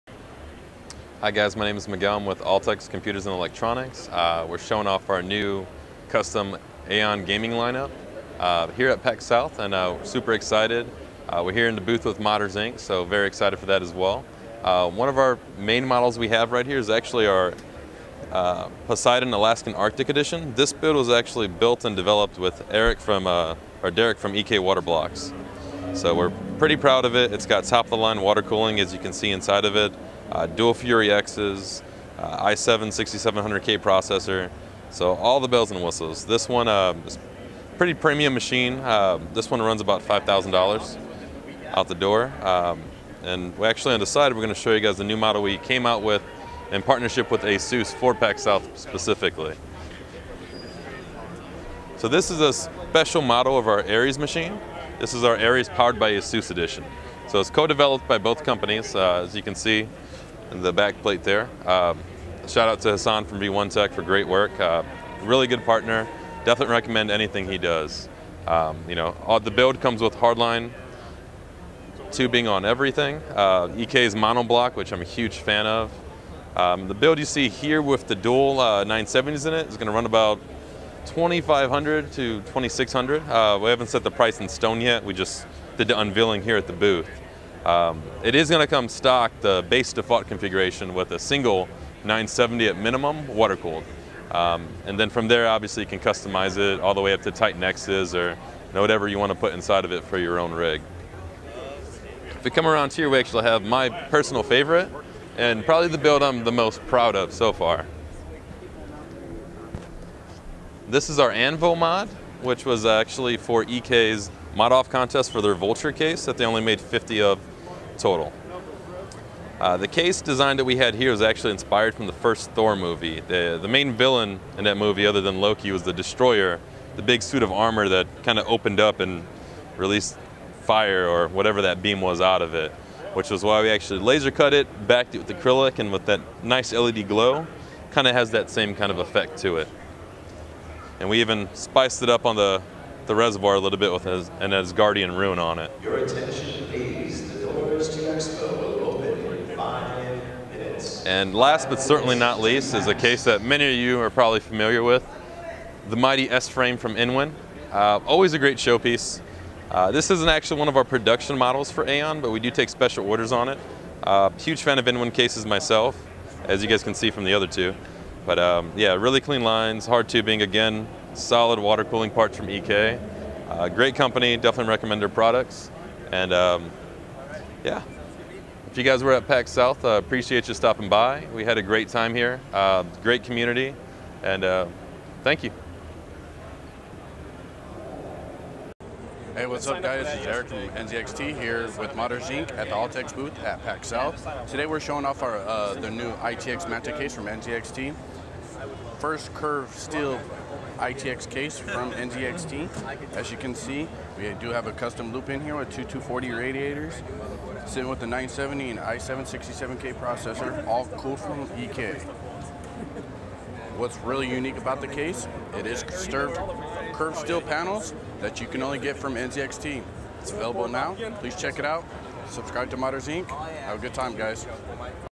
Altex Computers + NZXT Manta Case @ PAX South 2016
Miguel from Altex Computers gives a walkthrough of their latest AION Gaming PC lineup. Altex has partnered up with EKWB, ASUS and V1 Tech to create their premium custom gaming systems.

Also on display is a custom EK Vulture casemod. The EK Vulture is a limited edition compact tower case and only 50 pieces were made but EKWB has selected a few modders around to world to create custom mods for it upon release for promotion.

••• Support Modders Inc. while shopping at Amazon.com •••
Amazon US =

••• Follow Modders Inc. on Social Media •••